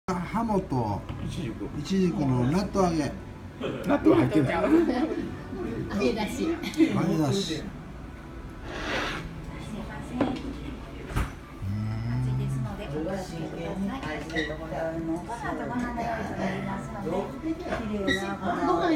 大阪の堺市北花田にある和食店 旬菜 箪ぽぽ すずらんのコースで全八品 3,800円 No5 割烹・小料理 創作料理 釜飯
旬菜　箪ぽぽ、
住所：大阪府　堺市　北区　北花田町3丁33－5
電話：072－250-6505
営業時間
昼　）11：30～14：00
夜　）17：30～22：00
金、土のみ23：00